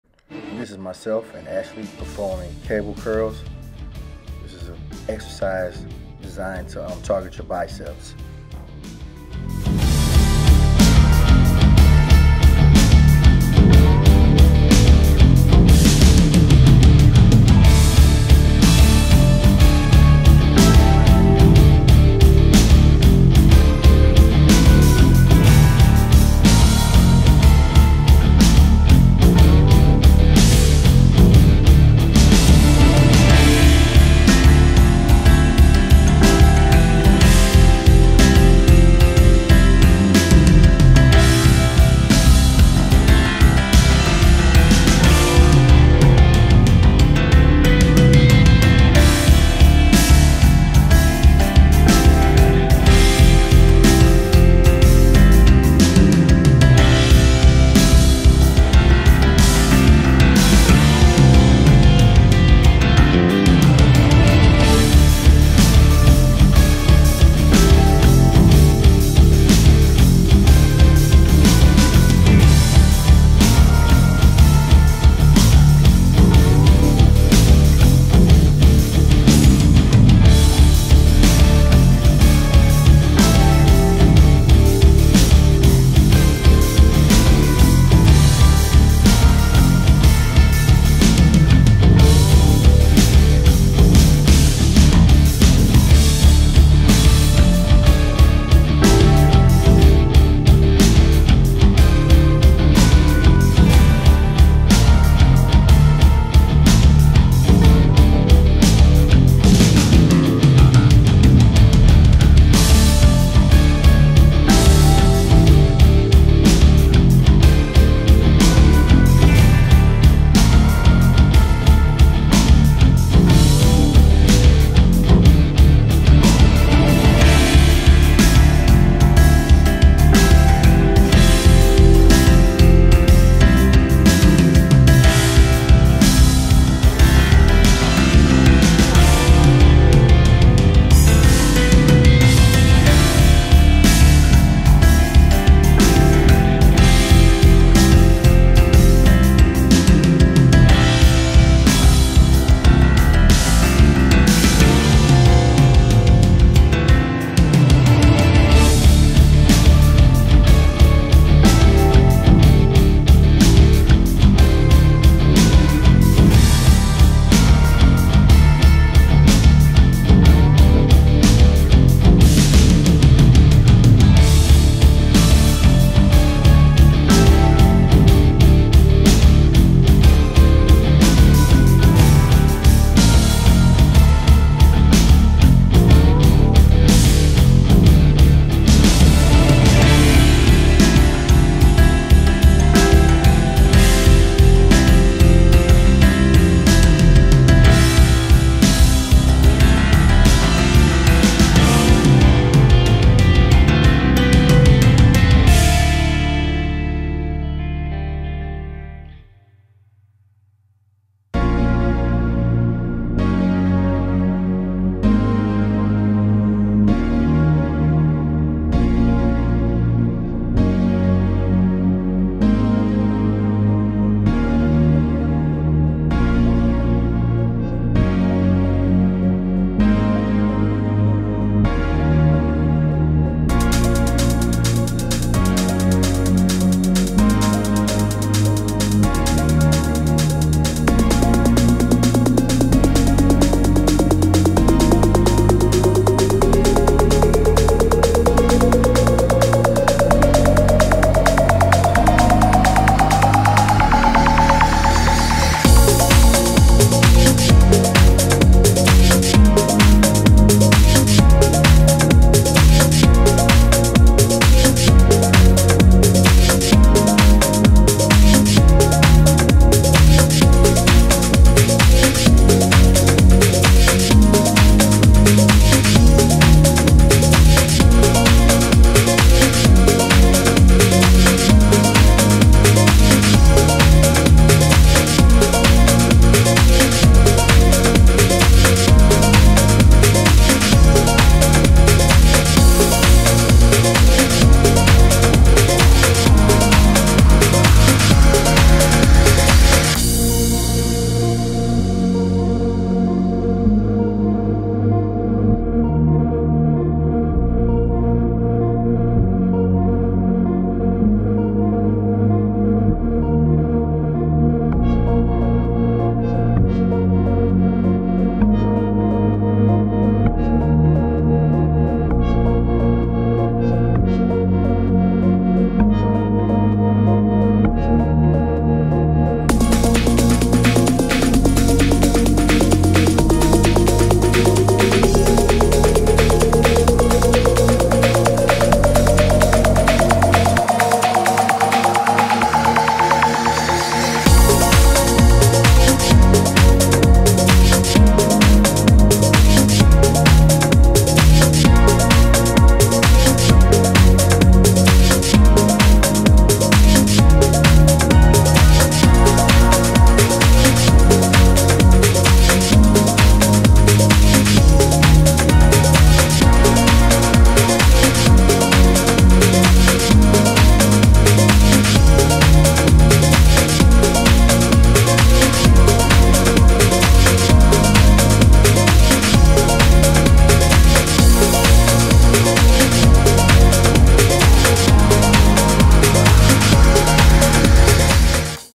AKR M E Fitness cable curls with Ashley SD 480p
Ashley and myself doing curls on the cable machine